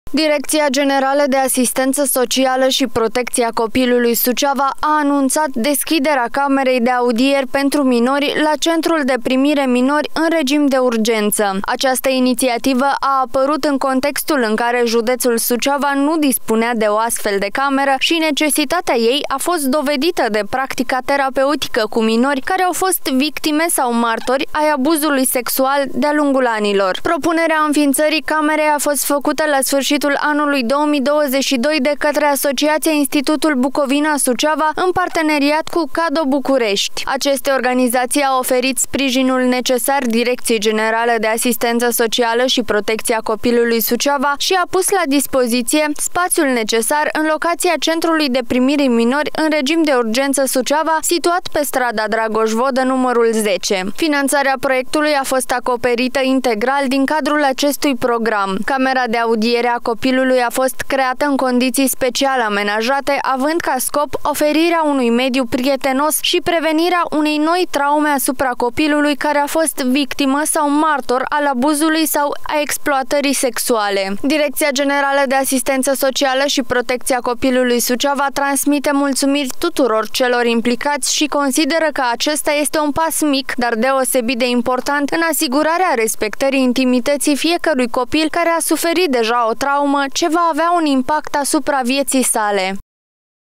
DGASPC SUCEAVA DESCHIDE PRIMA CAMERA DE AUDIERI PENTRU MINORI ÎN LUPTA ÎMPOTRIVA ABUZULUI SEXUAL
Direcția Generală de Asistență Socială și Protecția Copilului (DGASPC) Suceava a anunțat cu bucurie deschiderea primei Camere de audieri pentru minori de la Centrul de Primire Minori în Regim de Urgență din județ. Inițiativa înființării acestei camere a apărut din necesitatea de a oferi un mediu adecvat pentru minorii care au fost victime sau martori ai abuzului sexual.